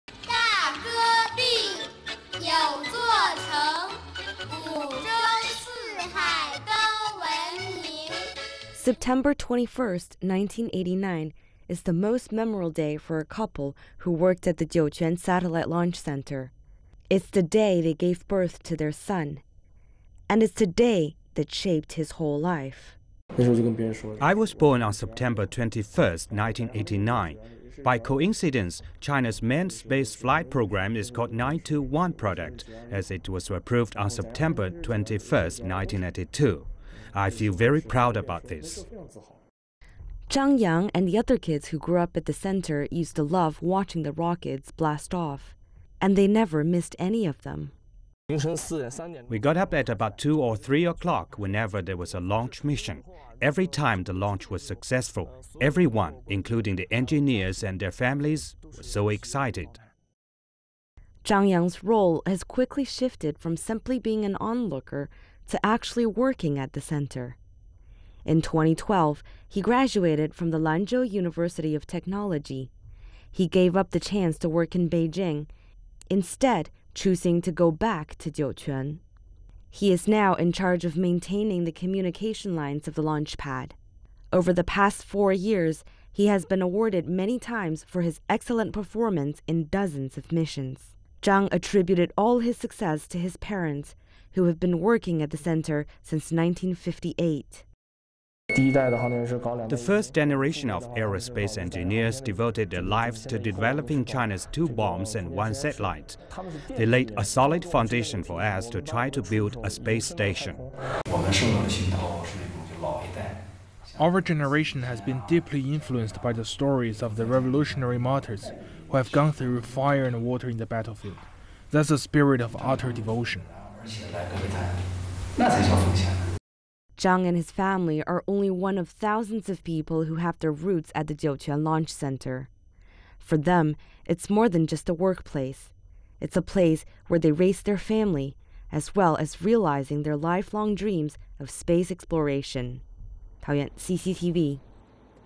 China's Jiuquan city: Home to generations of space engineers
People living at China's Jiuquan Satellite Launch Center have been witnesses to many of the most important moments in the history of China's space program. Some of them moved to the city for work, while later generations grew up and chose to remain, to continue their life-long dream of space exploration.